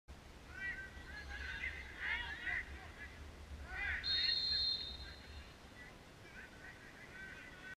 20141003 IFK Vuku GULT2#11 B
Oppgjør i 4.div Menn avd 01 - Trøndelag Fotballkrets 3.oktober 2014 mellom Innherred FK og Vuku.